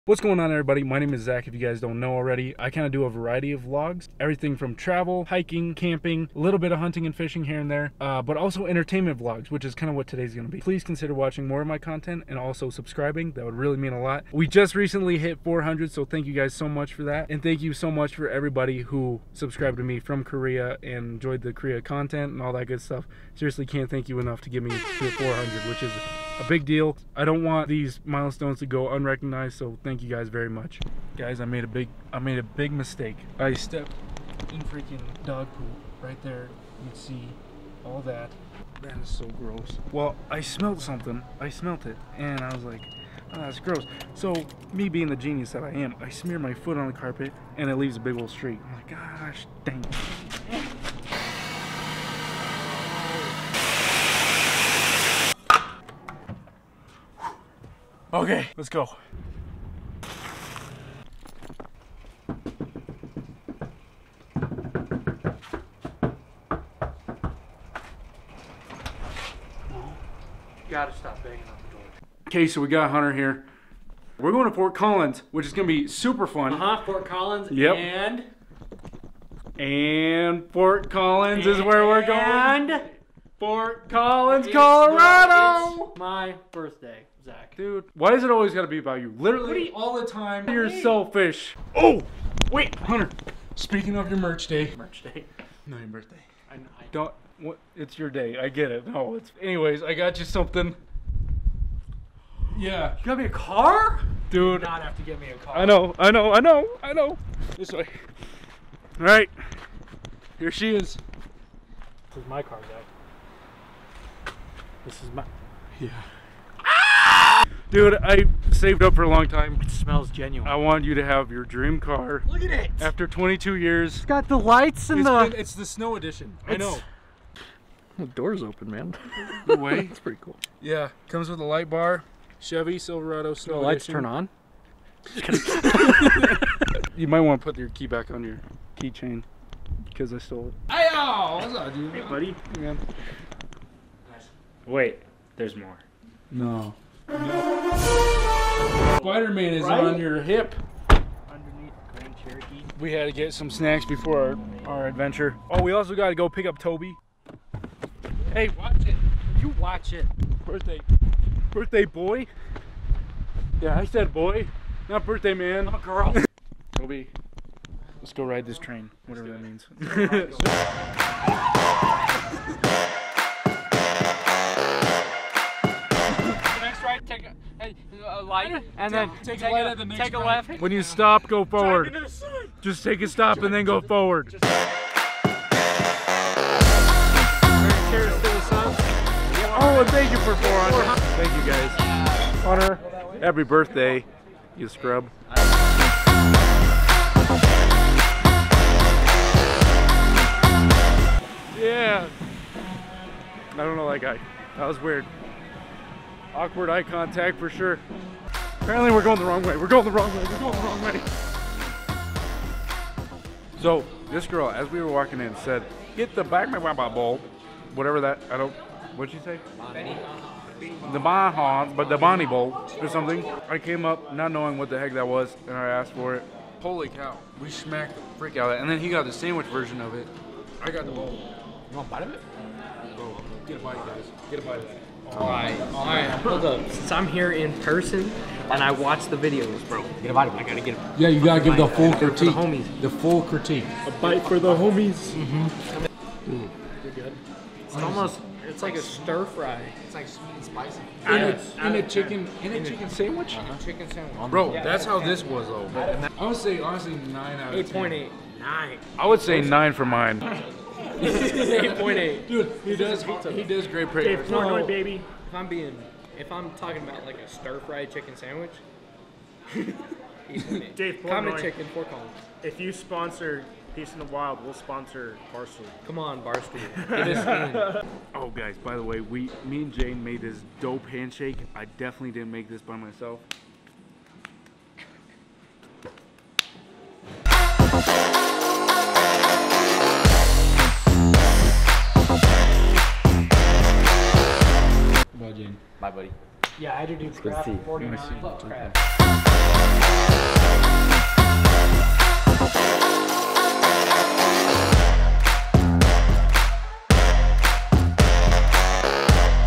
Hunter's Dream Truck and Birthday in Fort Collins CO | Bar Hopping, and Handshakes, and Ice Cream!
This is Hunter's Dream Truck and  Wild Birthday in Fort Collins, Colorado | Bar Hopping, Food, Handshakes, and Ice Cream! Join us for Hunter's unforgettable birthday celebration, with hunter dream truck. It wasn't easy but it was great to be able to buy friend his dream truck after all these years. in Fort Collins, Colorado! We take you on a wild journey through some of Fort Collins hottest bars, indulging in delicious food and drinks. You'll also witness the creation of our epic handshake, which we practiced all night long! To top it all off, we end the night with a sweet treat of creamy, decadent ice cream at one of Fort collins best ice cream shops. Don't miss out on this crazy adventure filled with laughter, good times, and great memories!
00:00 Introduction
00:34 dog poop
00:59 Hunter's dream truck
02:18 Travel to Fort Collins
03:08 Fort Collins Celebration
04:53 outro